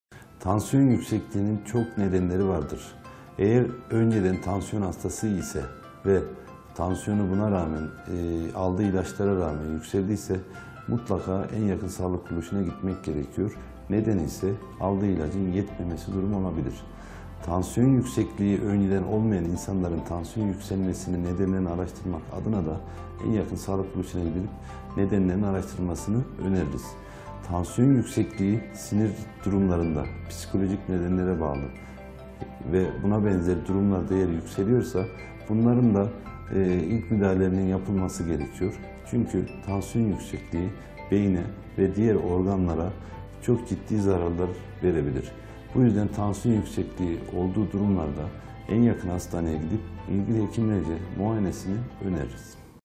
Tansiyonum Yükseldi Ne Yapmalıyım?
Tansiyonum Yükseldi Ne Yapmalıyım? Acil Servis Uzmanı Dr. Erol KARAKAŞ videomuzda yanıtlıyor.

38001668